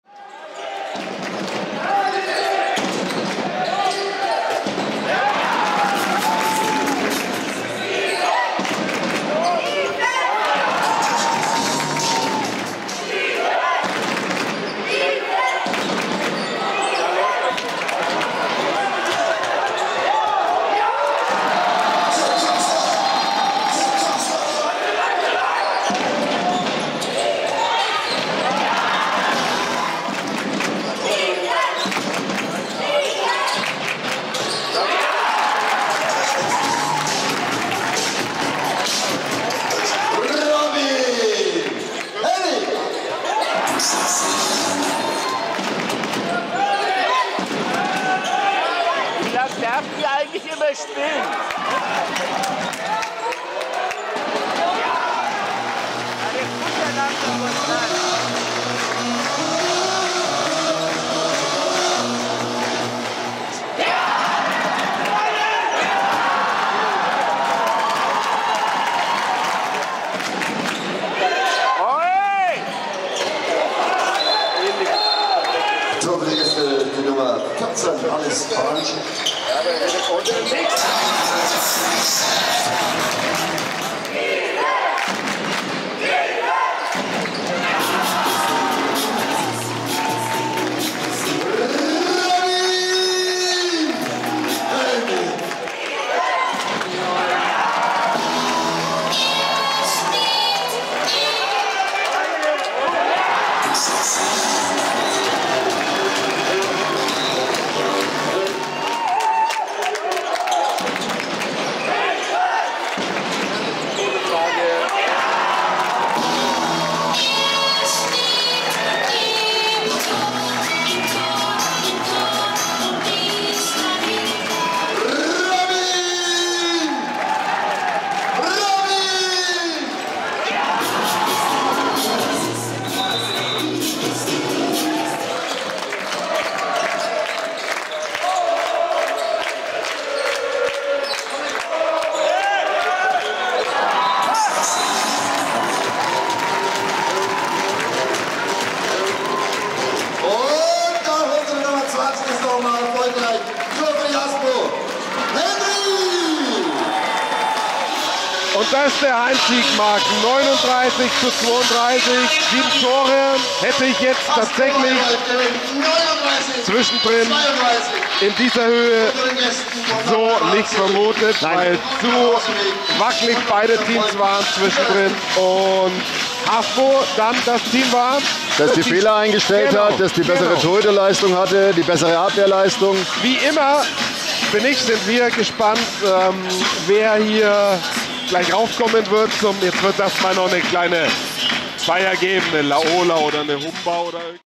Handball ist cool: Tolle Paraden und 3 Torwart-Tore von Robin Henning (HaSpo – NHC, 11.02.2023) …
… beim Spiel der 3.Liga Ost HaSpo Bayreuth – Northeimer HC. Und hier ist er anschließend im Interview mit Dirk Feustel
"Die volle Action der 3. Liga Männer LIVE und exklusiv auf @Sportdeutschland.TV DHB_Liga3"